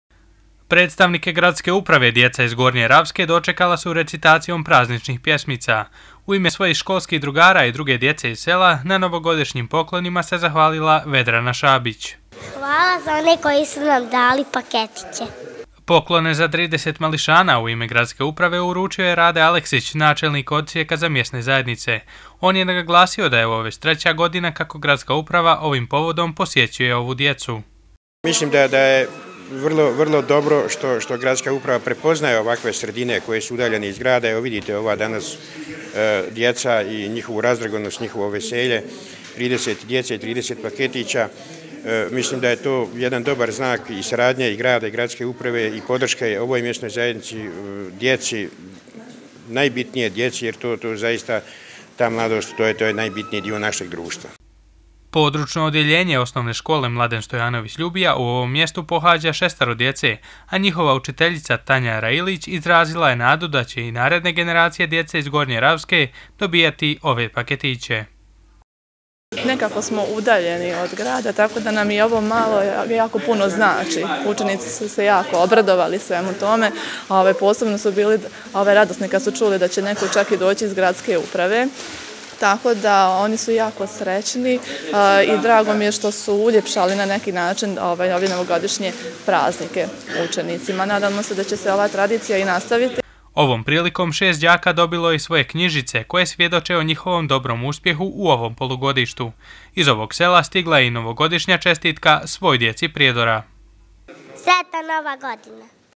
NOVOGODŠINJI POKLONI ZA DJECU GORNJE RAVSKE
Predstavnici Gradske uprave posjetili su danas Gornju Ravsku, i tom prilikom uručili novogodišnje paketiće za 30 djece koja žive u ovom selu. Poklone im je uručio Rade Aleksić, šef Odsjeka za mjesne zajednice grada Prijedora, koji je napomenuo da je ovo već treća godina kako se na ovaj način uljepšavaju praznici mališanima koji žive u jednom od najudaljenijih prijedorskih sela.